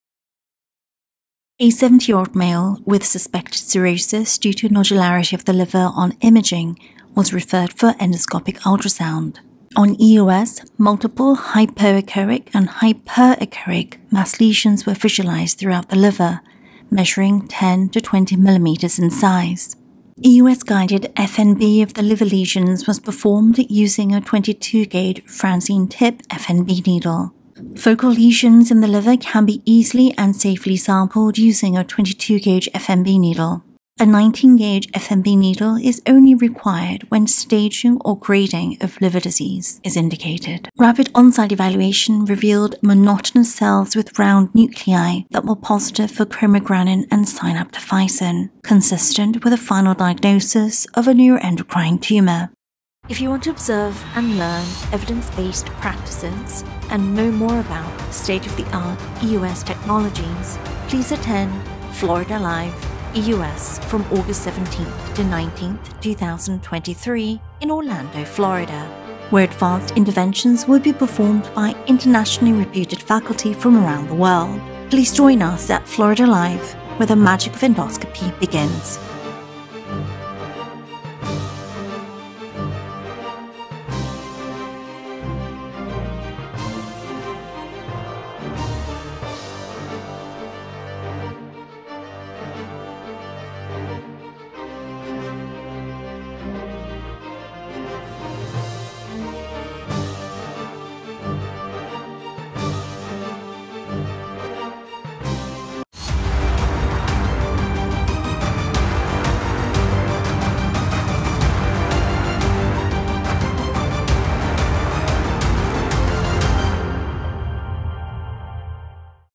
Florida Live EUS Educational Video (August 17-19, 2023) - Liver mass in liver cirrhosis
We are pleased to present this case vignette of a patient with chronic liver disease and suspected multinodular liver cirrhosis on CT scan. At EUS-guided liver biopsy, the diagnosis was malignancy but not the kind one would expect.

To observe such interesting cases, meet with your peers, and learn the latest advances in EUS, please attend Florida Live Endoscopy from August 17-19, 2023 in Orlando where the Magic of Endoscopy begins!